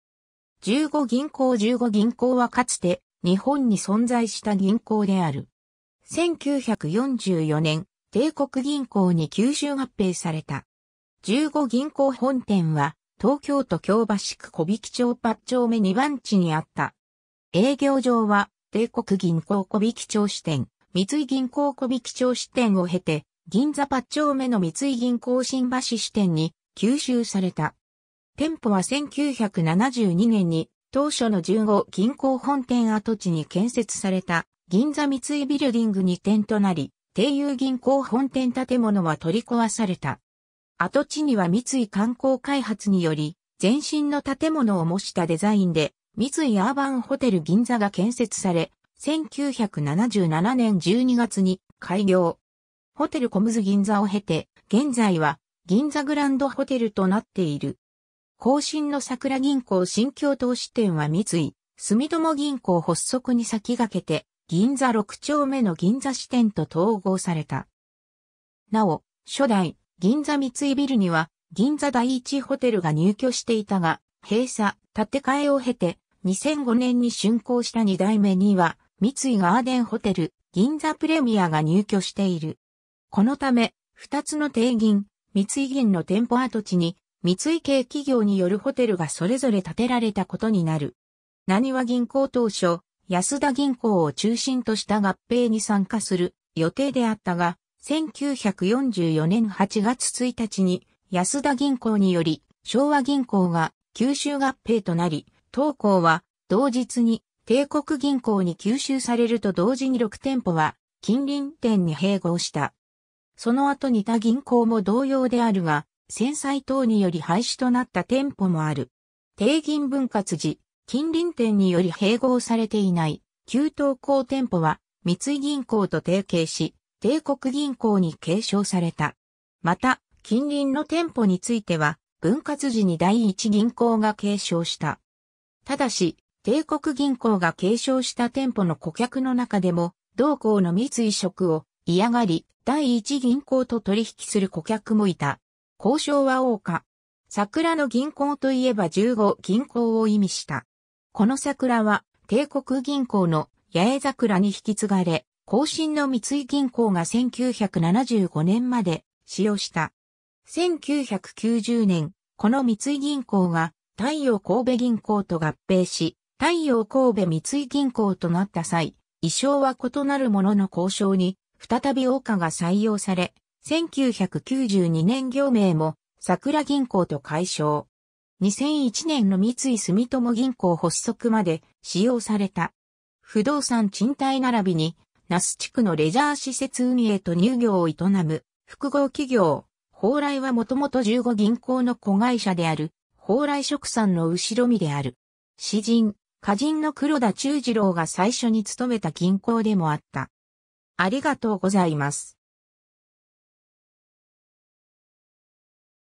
十五銀行
十五銀行 十五銀行（じゅうごぎんこう）はかつて日本に存在した銀行である。
1944年、帝国銀行に吸収合併された。
 十五銀行本店は東京都京橋区木挽町八丁目2番地（現在の中央区銀座八丁目）にあった。
 営業上は帝国銀行木挽町支店→三井銀行木挽町支店を経て、銀座八丁目の三井銀行新橋支店（旧丁酉銀行本店→十五銀行新橋支店→帝国銀行新橋支店）に吸収された。
店舗は1972年に当初の十五銀行本店跡地に建設された銀座三井ビルディング（初代）に移転となり、丁酉銀行本店建物は取り壊された。
跡地には三井観光開発（2007年よりグランビスタホテル&リゾート）により、前身の建物を模したデザインで「三井アーバンホテル銀座」が建設され、1977年12月に開業。
「ホテルコムズ銀座」を経て現在は「銀座グランドホテル」となっている。
 後身のさくら銀行新橋東支店は三井住友銀行発足に先駆けて、銀座六丁目の銀座支店と統合された。
なお、初代・銀座三井ビルには銀座第一ホテルが入居していたが、閉鎖・建て替えを経て、2005年に竣工した2代目には三井ガーデンホテル銀座プレミアが入居している。
このため、2つの帝銀・三井銀の店舗跡地に三井系企業によるホテルがそれぞれ建てられた事になる。
 浪速銀行 当初安田銀行を中心とした合併に参加する予定であったが、1944年8月1日に安田銀行により昭和銀行が吸収合併となり、当行は同日に帝国銀行に吸収されると同時に6店舗は近隣店に併合した。
 その後に他銀行も同様であるが、戦災等により廃止となった店舗もある。
 帝銀分割時、近隣店により併合されていない旧当行店舗は、三井銀行と提携し（新）帝国銀行（後の三井銀行）に継承された。
また、近隣の店舗については分割時に（新）第一銀行が継承した。
 ※ただし、（新）帝国銀行が継承した店舗の顧客の中でも同行の三井色を嫌がり、第一銀行と取引する顧客もいた。
 行章は桜花。
「桜の銀行」といえば十五銀行を意味した。
この桜は帝国銀行の「八重桜」に引き継がれ、後身の三井銀行が1975年まで使用した。
1990年、この三井銀行が太陽神戸銀行と合併し太陽神戸三井銀行となった際、意匠は異なるものの行章に再び桜花が採用され、1992年行名もさくら銀行と改称。
2001年の三井住友銀行発足まで使用された。
 不動産賃貸並びに那須地区のレジャー施設運営と乳業を営む複合企業、ホウライは元々十五銀行の子会社である蓬莱殖産の後身である。
 詩人・歌人の黒田忠次郎が最初に勤めた銀行でもあった。